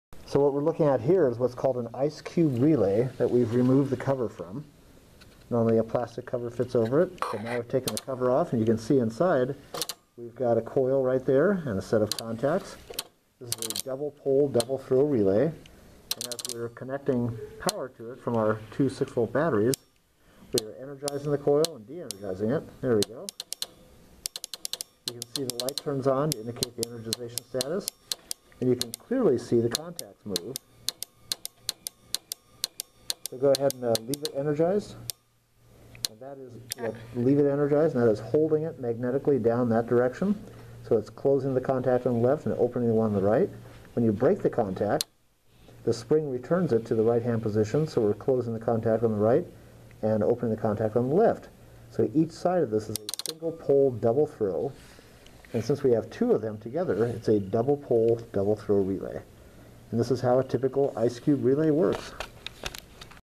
Ice cube relay demonstration
Simple demonstration of an "ice cube" style relay with the cover removed.